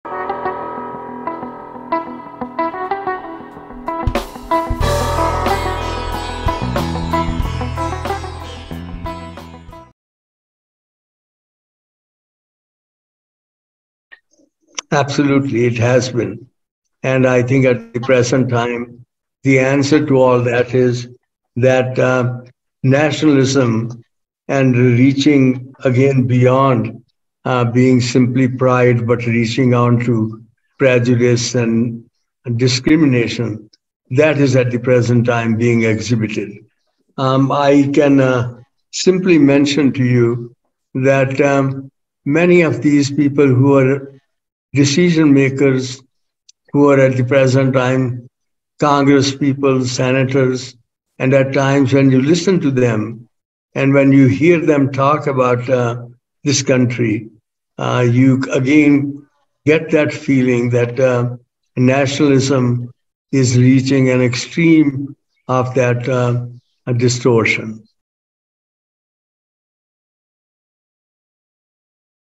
What is the impact of exercising Religion and Spirituality in the name of Nationalism ?
Bharatiya Vichar Manch
Indo American Intellectuals Forum, Los Angeles

Topic: Good Nationalism v/s Bad Nationalism and where does Hindu Nationalism fit in? Lecture by Padma Bhushan Prof. Ved Nanda

BVM Mission: To Promote dialogue about Dharmic philosophy culture and traditions which can positively influence global thoughts and actions.

About Speaker : Padma Bhushan Prof. Ved Nanda serves as the Chairman of the Board of Hindu University of America. Dr. Nanda is a Professor of Law at the Sturm College of Law, University of Denver and serves as Director Emeritus of the International Legal Studies Program there. In 2006, Prof. Nanda was honored with the founding of the Nanda Center for International Law. He served as Vice Provost at the University of Denver from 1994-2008. He holds or has held numerous official posts in international, regional, and national professional and civil society organizations. He is widely published, having authored or co-authored 24 books in various fields of international law. Prof. Ved Nanda has also received a number of awards including the Gandhi, King, Ikeda Award for Community Peace Building.